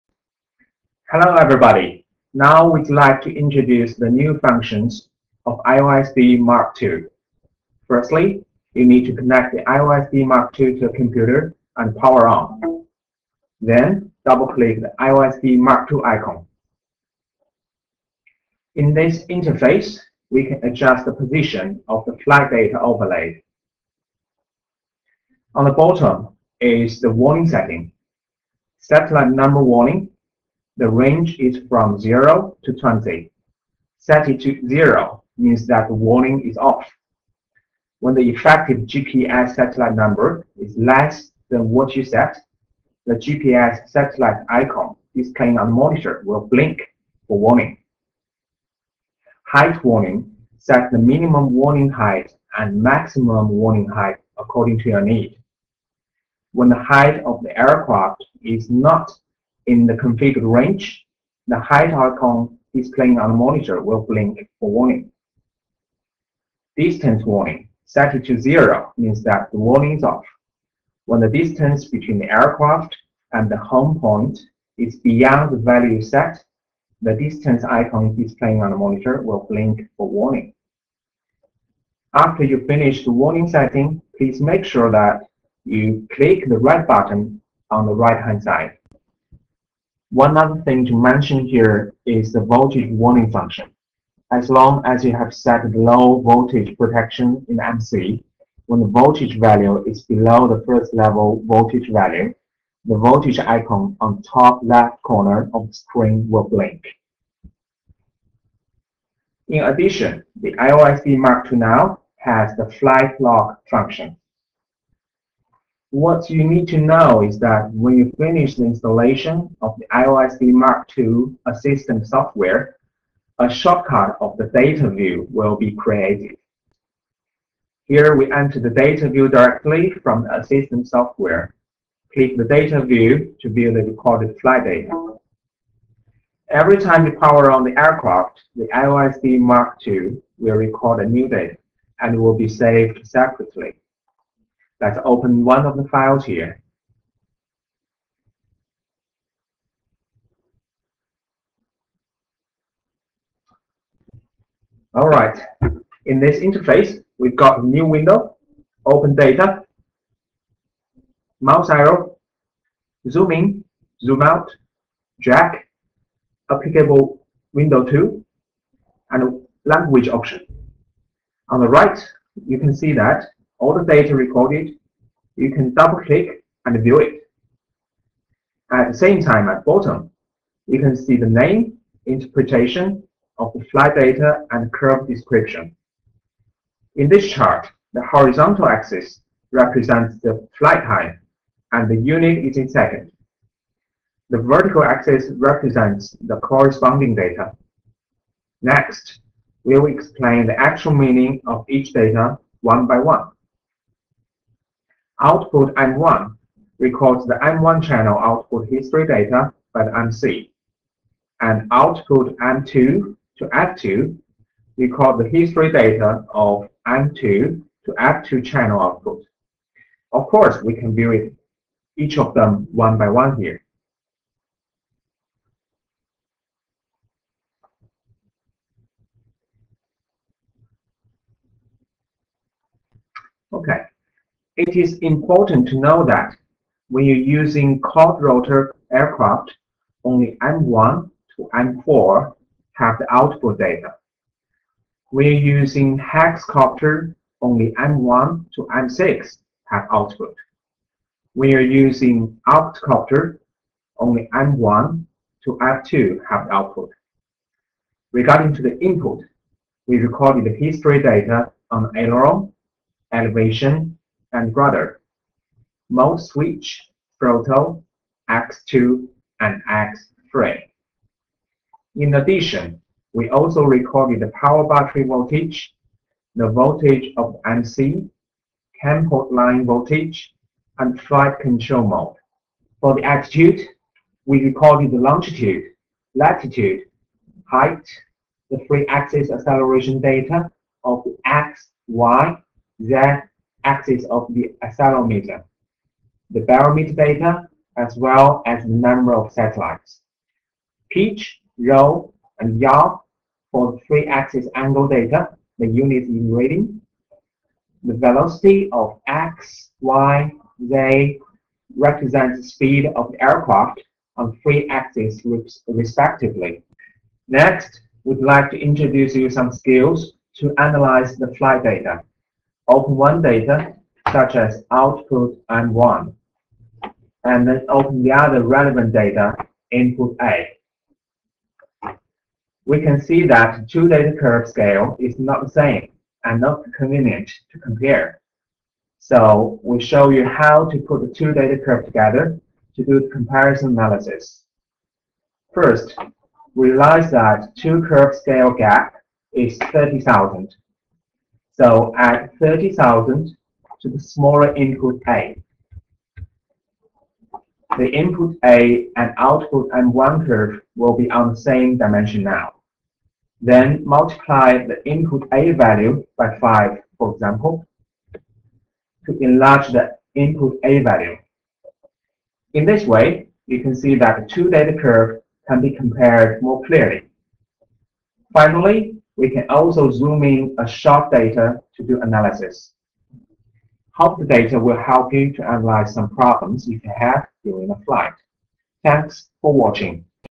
DJI iOSD Mark II Assistant Software Introduction
DJI iOSD is specially designed for DJI autopilot system duringthe FPV flight or other aero-modeling activates. DJI iOSDcan transmit video and iOSD information in real time, which will help you to obtain the aircraft status information during a FPV flight. It can display power voltage, flight velocity, height, distance from the home point, horizontal attitude, GPS satellite number, etc. iOSD and video information are superposed on the receiver, making iOSD data clearly visible and bringing you a more involved flight experience.